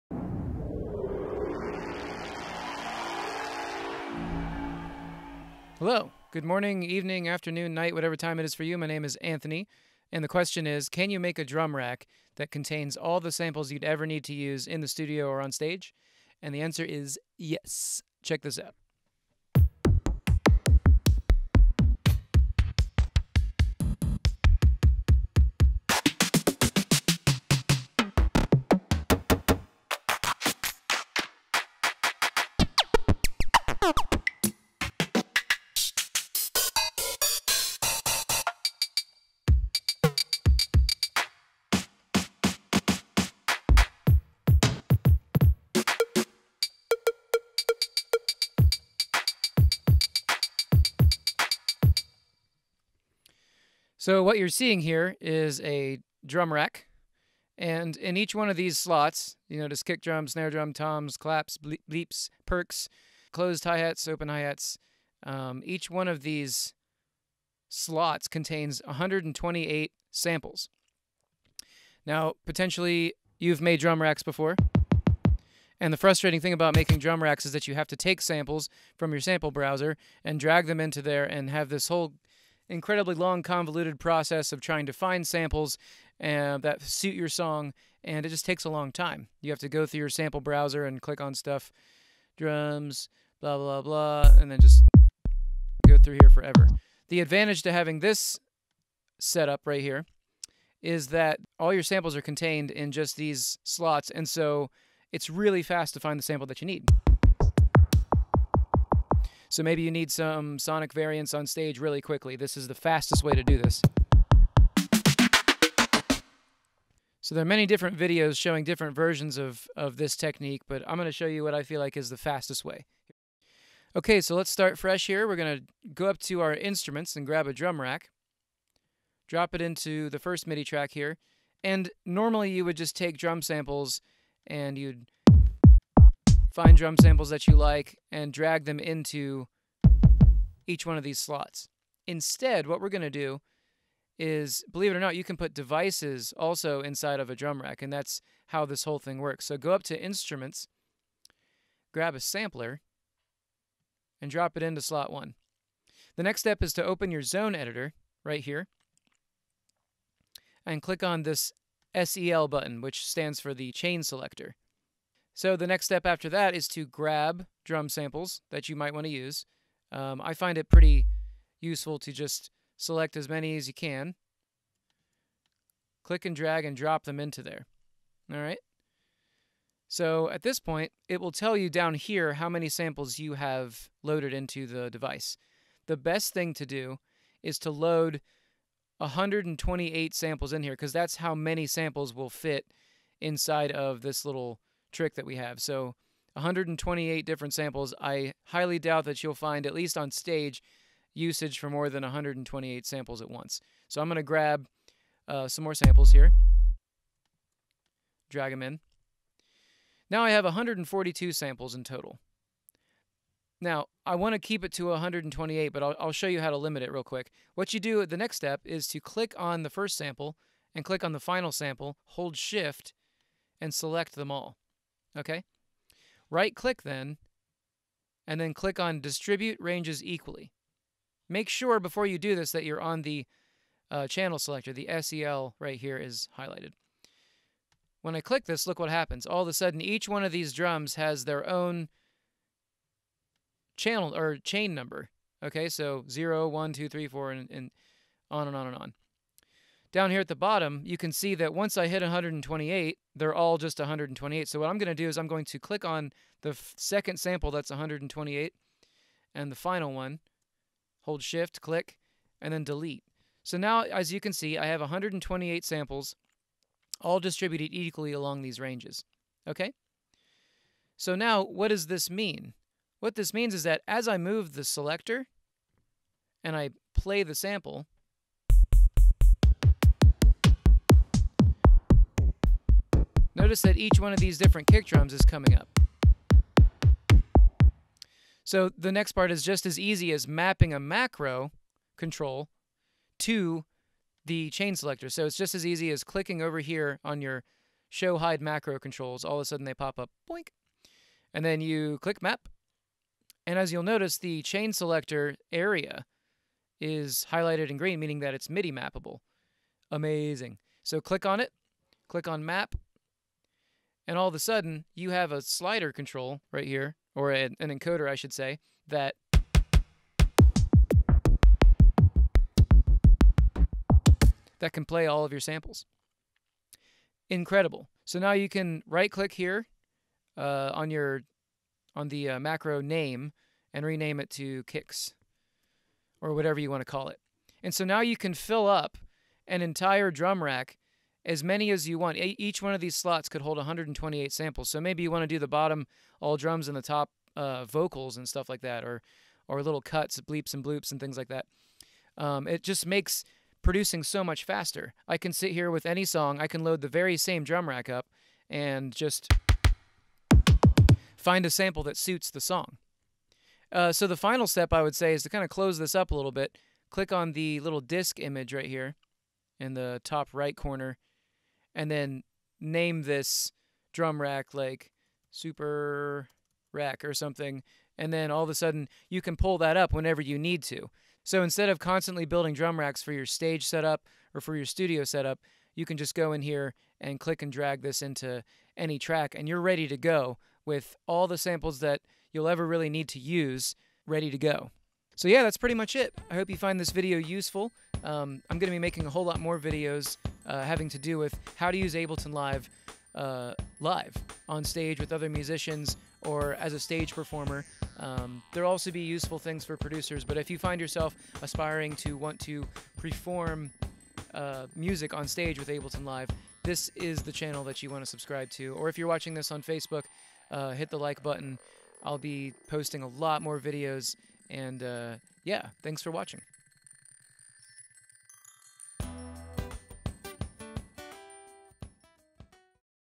All Your Favorite Samples in a Single Drum Rack!?
Collecting your samples into Ableton Drum Racks each time you want to create a new beat can be a royal pain. In this quick little video, I'll show you how to create a drum rack where each slot can contain up to 128 samples! Creating this drum rack can save you lots of time in the studio and keep your live show interesting and evolving.